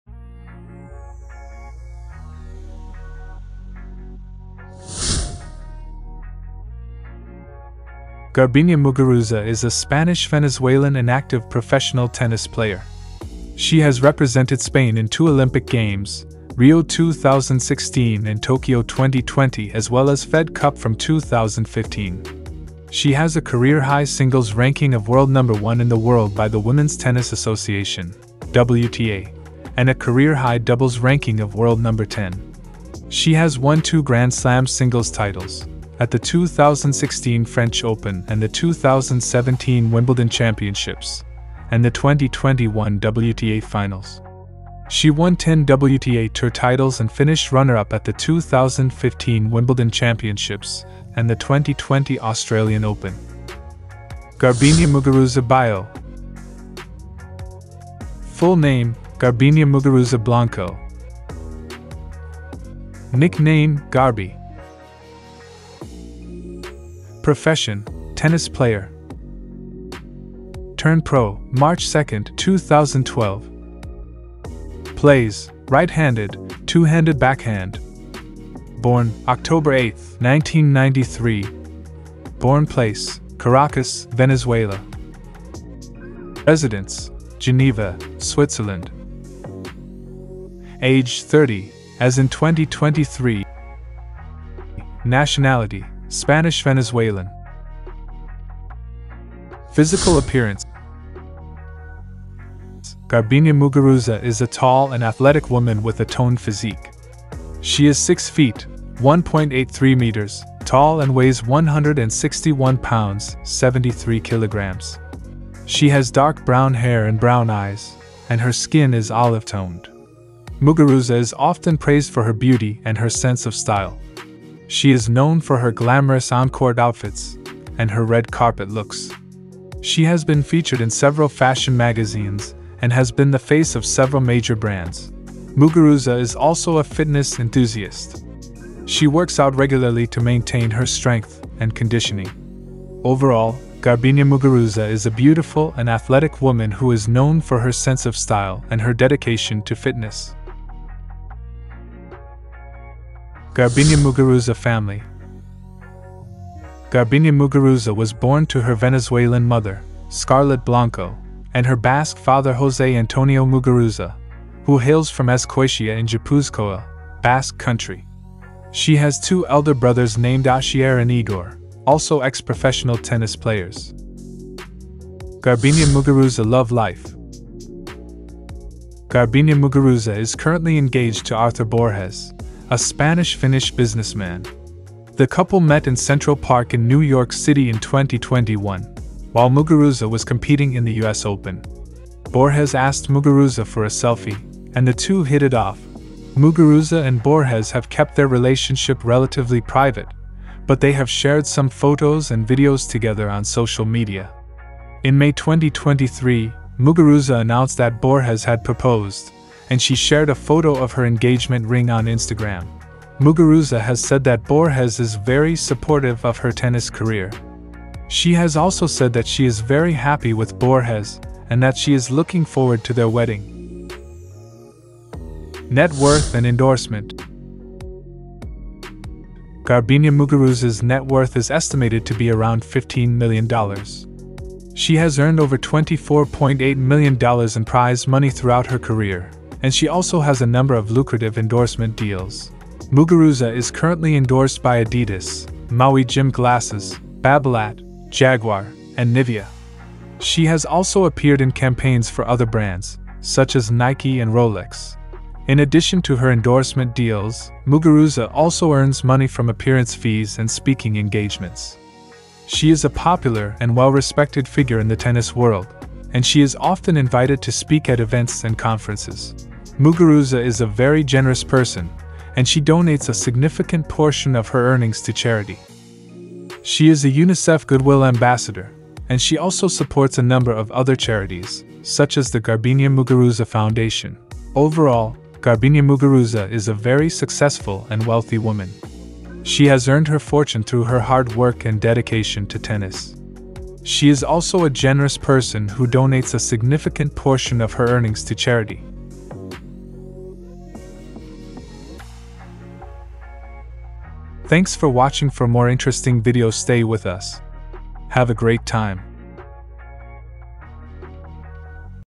Garbiñe Muguruza Biography, Family, Parents, Love Life, Partner, Net Worth & Lifestyle
Garbiñe Muguruza is a Spanish-Venezuelan inactive professional tennis player. She has represented Spain in two Olympic Games, Rio 2016 and Tokyo 2020 as well as Fed cup from 2015. She has a career-high singles ranking of world No. 1 in the world by the Women's Tennis Association (WTA), and a career-high doubles ranking of world No. 10. She has won two Grand Slam singles titles, at the 2016 French Open and the 2017 Wimbledon Championships, and the 2021 WTA Finals. She won ten WTA Tour titles and finished runner-up at the 2015 Wimbledon Championships and the 2020 Australian Open.

Muguruza is a popular and well-respected figure in the tennis world. She is known for her aggressive playing style and her powerful serve. She is also a fan favorite for her outgoing personality and her sense of humor.

Garbiñe Muguruza Bio:

Full Name: Garbiñe Muguruza Blanco.
Nick Name: Garbi.
Profession: Tennis Player.
Turned pro : 2 March 2012.
Plays: Right-handed (two-handed backhand)
Born: 8 October 1993
Born Place: Caracas, Venezuela.
Residence: Geneva, Switzerland.
Age: 30 (as in 2023)
Nationality: Spanish-Venezuelan.
Religion: Christian.

Garbiñe Muguruza Physical Appearance:

Garbiñe Muguruza is a tall and athletic woman with a toned physique. She is 6 feet (1.83 meters) tall and weighs 161 pounds (73 kilograms). She has dark brown hair and brown eyes, and her skin is olive-toned.

Muguruza is often praised for her beauty and her sense of style. She is known for her glamorous on-court outfits and her red carpet looks. She has been featured in several fashion magazines and has been the face of several major brands.

Muguruza is also a fitness enthusiast. She works out regularly to maintain her strength and conditioning.

Overall, Garbiñe Muguruza is a beautiful and athletic woman who is known for her sense of style and her dedication to fitness.

Garbiñe Muguruza Family:

Garbiñe Muguruza was born to her Venezuelan mother, Scarlet Blanco, and her Basque father José Antonio Muguruza, who hails from Azkoitia in Gipuzkoa, Basque Country. She has two elder brothers named Asier and Igor, also ex-professional tennis players, and has dual Spanish-Venezuelan citizenship. Muguruza began playing tennis at the age of three at the tennis courts of Mampote, in Guarenas outside Caracas, and already showed great talent and skill.

After moving to Spain with her family when she was six years old, looking for better hopes in her tennis career, Muguruza began training at the Bruguera Tennis Academy in Barcelona with her brothers, all dreaming of becoming professional tennis players.

Garbiñe Muguruza Love Life:

Garbiñe Muguruza is currently engaged to Arthur Borges, a Spanish-Finnish businessman. The couple met in Central Park in New York City in 2021, while Muguruza was competing in the US Open. Borges asked Muguruza for a selfie, and the two hit it off.

Muguruza and Borges have kept their relationship relatively private, but they have shared some photos and videos together on social media. In May 2023, Muguruza announced that Borges had proposed, and she shared a photo of her engagement ring on Instagram.

Muguruza has said that Borges is very supportive of her tennis career. She has also said that she is very happy with Borges and that she is looking forward to their wedding.

Garbiñe Muguruza Net Worth And Endorsement:

Garbiñe Muguruza's net worth is estimated to be around $15 million. She has earned over $24.8 million in prize money throughout her career, and she also has a number of lucrative endorsement deals.

Muguruza is currently endorsed by Adidas, Maui Jim glasses, Babolat, Jaguar, and Nivea. She has also appeared in campaigns for other brands, such as Nike and Rolex.

In addition to her endorsement deals, Muguruza also earns money from appearance fees and speaking engagements. She is a popular and well-respected figure in the tennis world, and she is often invited to speak at events and conferences.

Overall, Garbiñe Muguruza is a very successful and wealthy woman. She has earned her fortune through her hard work and dedication to tennis. She is also a generous person who donates a significant portion of her earnings to charity.